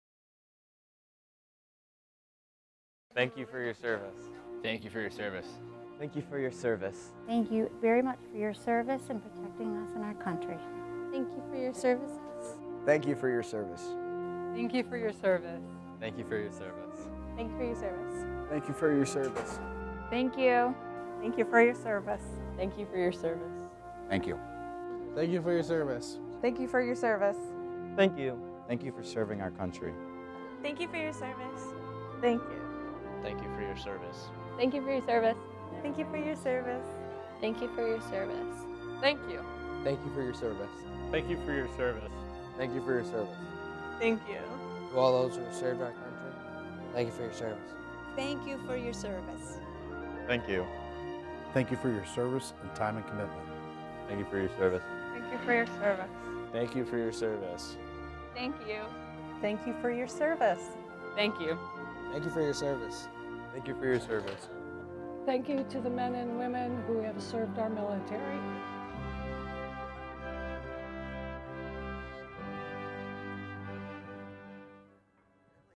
Valentines for Veterans
With Corning-Painted Post schools once again making Valentine's Day greetings for our Veterans at the Veterans Affairs center in Bath, NY, we wanted to take an opportunity to say, "Thank you."

Shot by: Gautam Vemury and Megan Rathbun
Edited by: Gautam Vemury